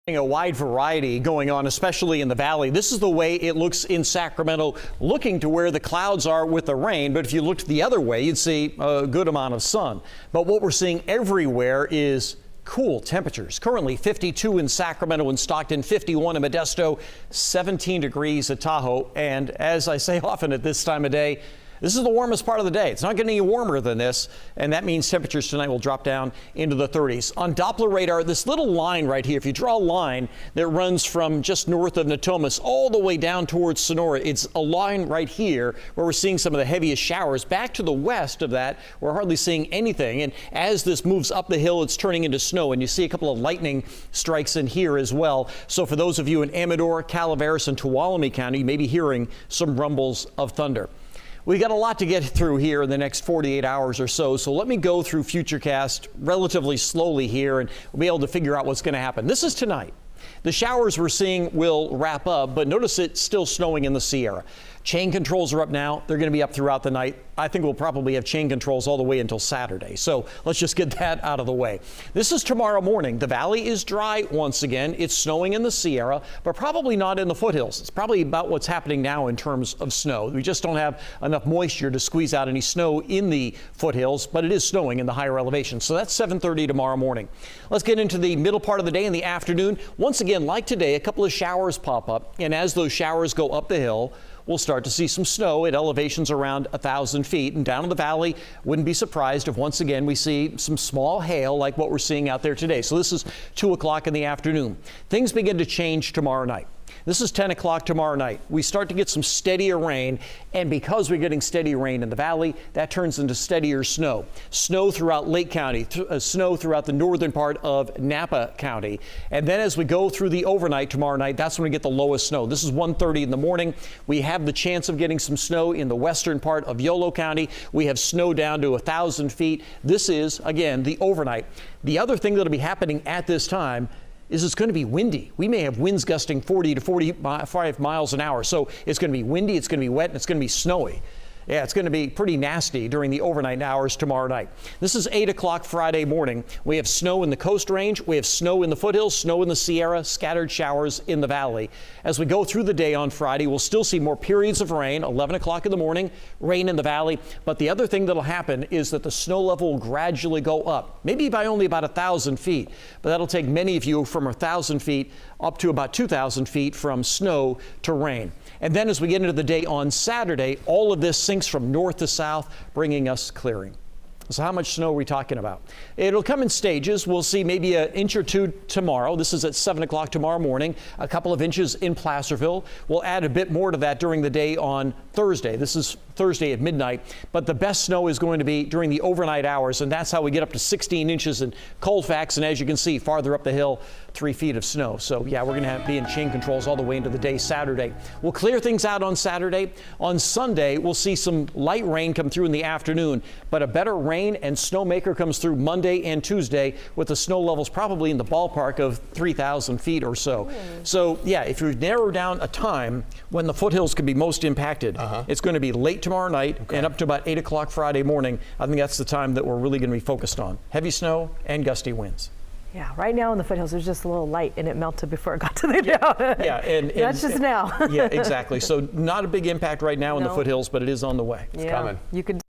When We'll See Low Snow in Northern California
Low snow will impact the area Thursday night and Friday morning